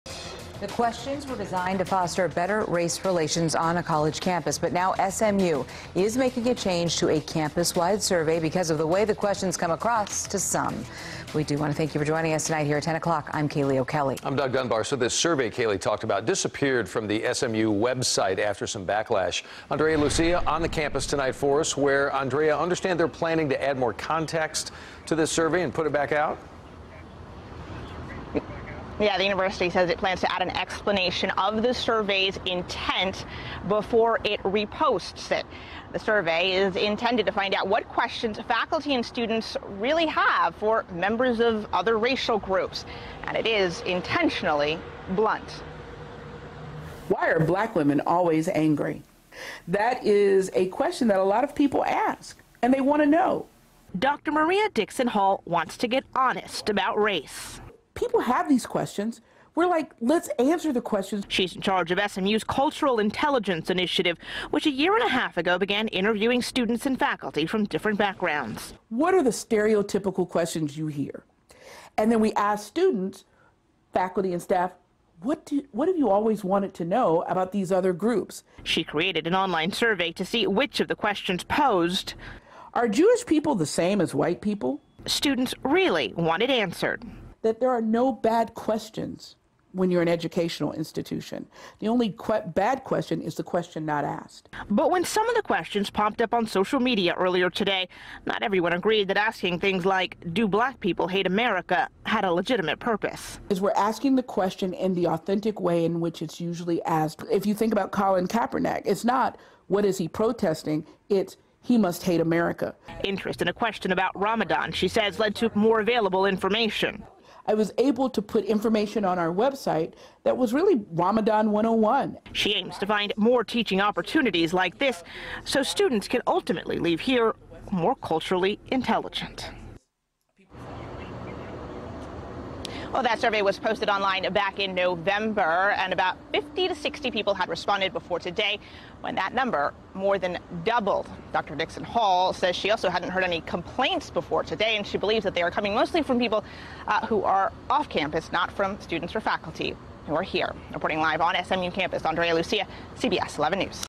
SMU Survey On Racial Stereotypes Taken Down After Backlash
The university says it plans to add more context to it's online survey.